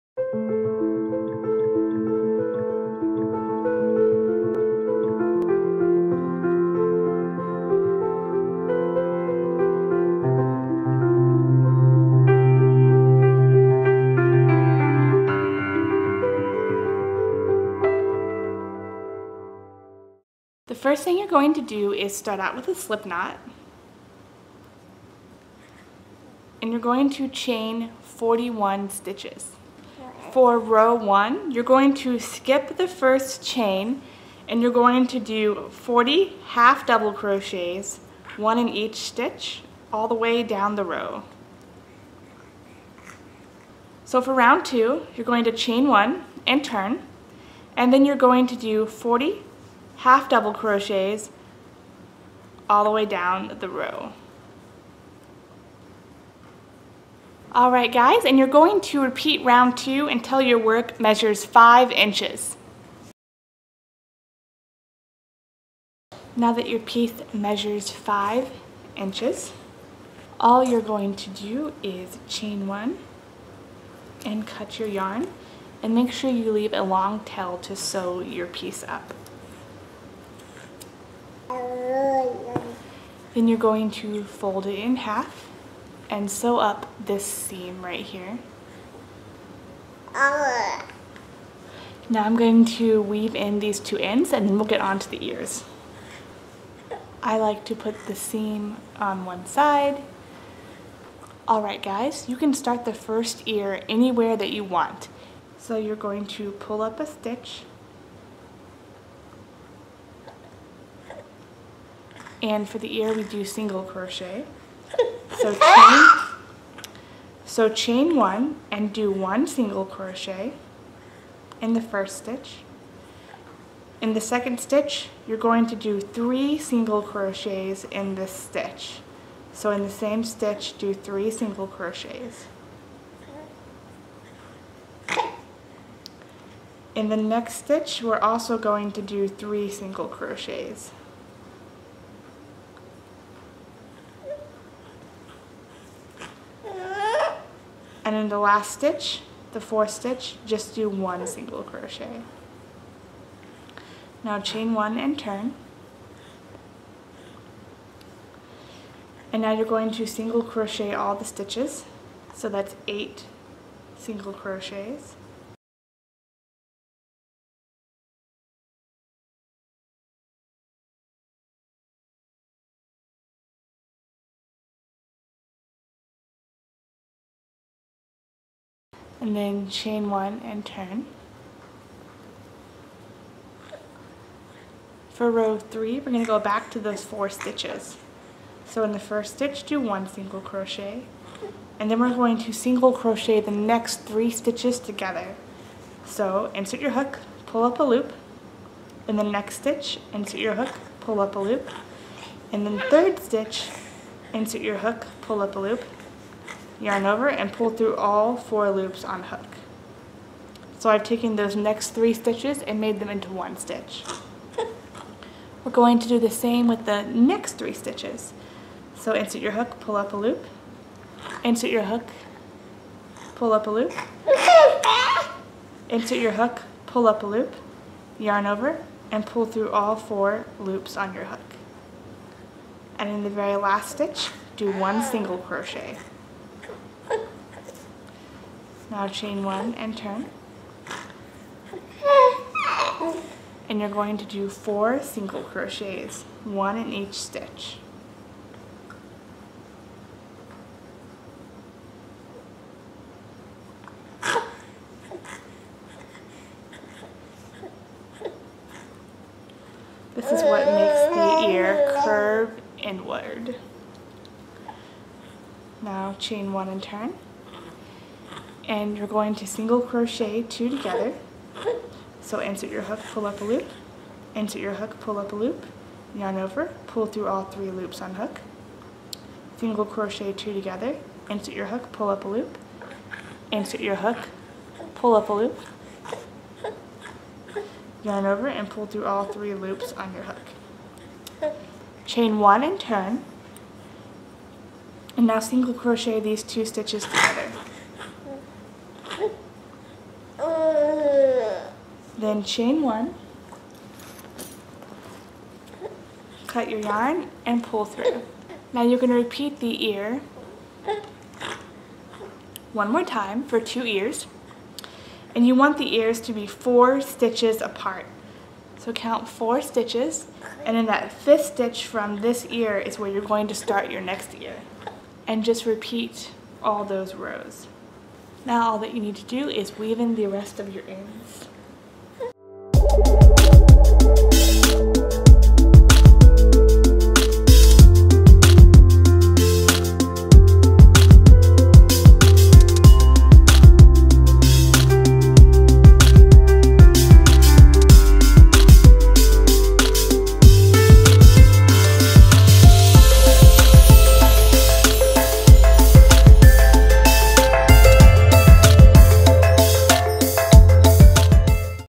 Crochet Cat Bootcuffs
Cute little crochet cat ear leg warmers for fall!

Skype me: @silver.moon88

I put out two videos a week, usually on Tuesdays and Thursdays!